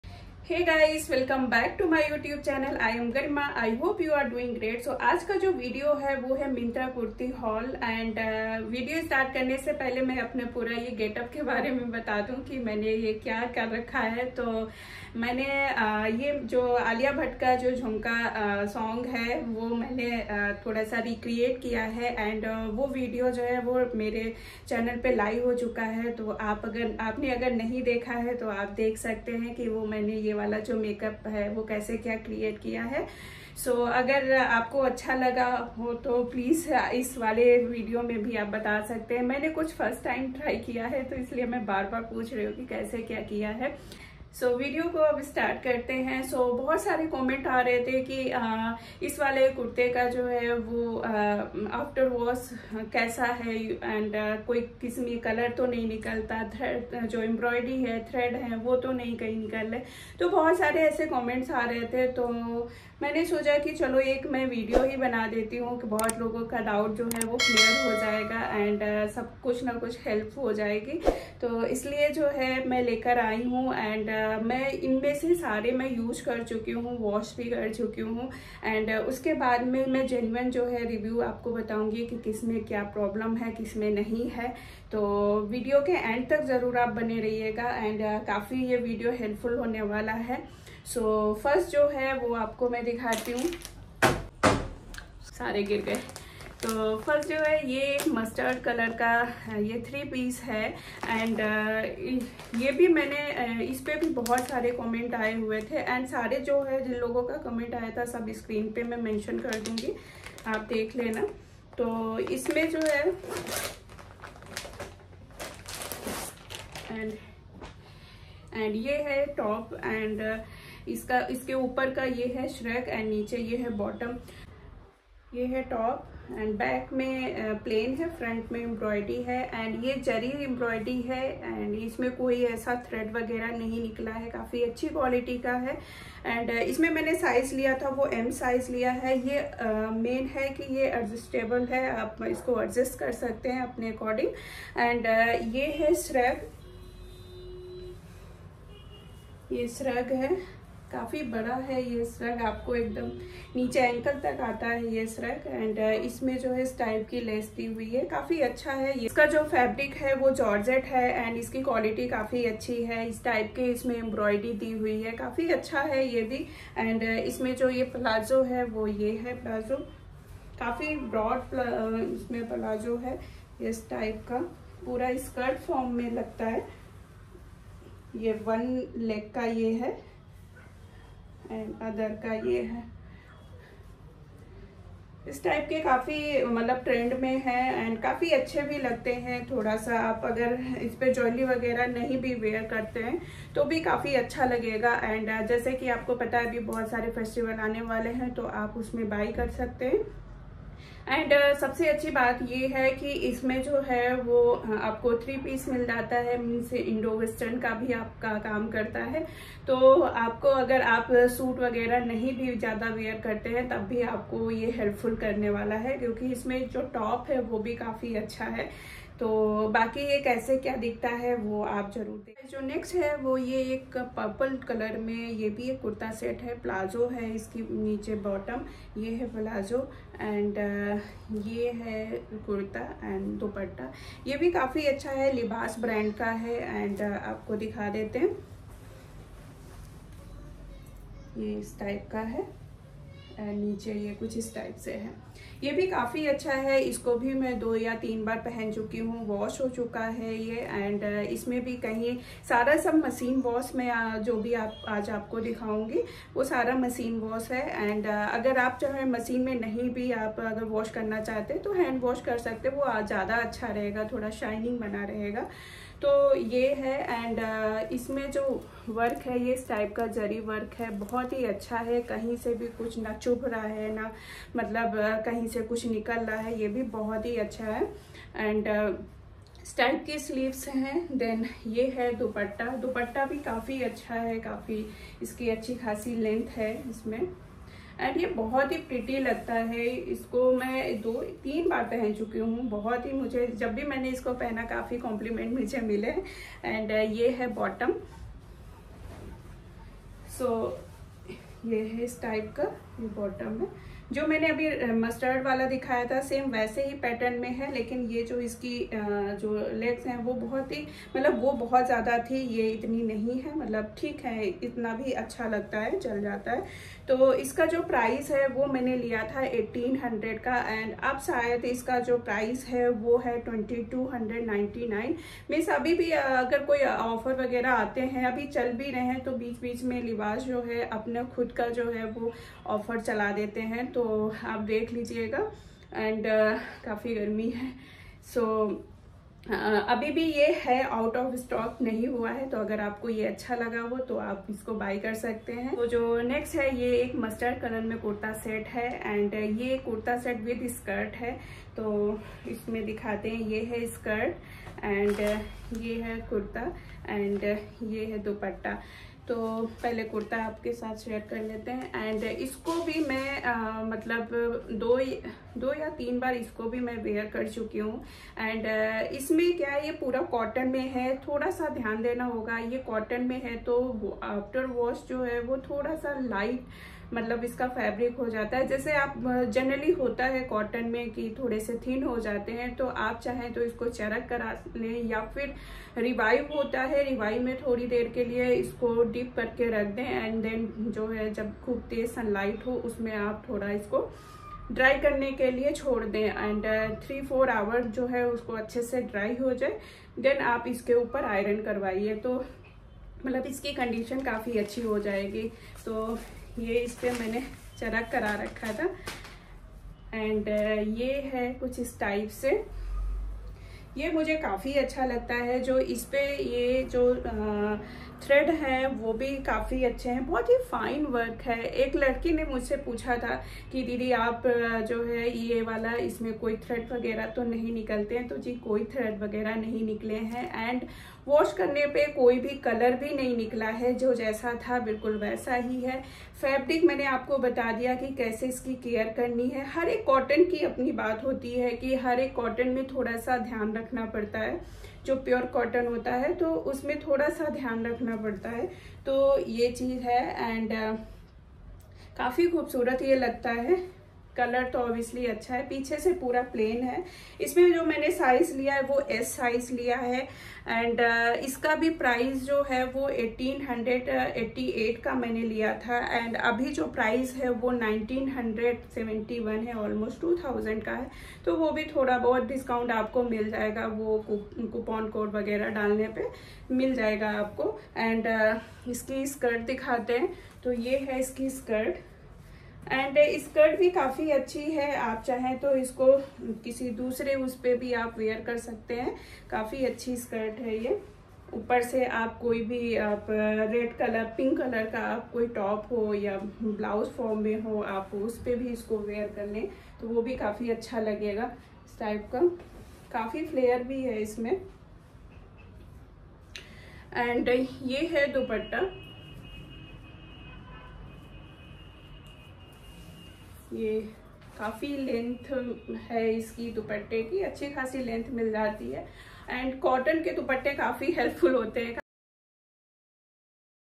Myntra kurti sets haul || After wash Honest reviews || Libas festive sale up to 70%
Myntra kurta sets haul

Ishin Women Yellow & Red Embroidered Kurta with Skirt & Dupatta

Kvsfab Women Elegant Mustard Self-Design Top with Solid Palazzos

Libas Women Floral Silk Blend Hyper Texture Kurta Set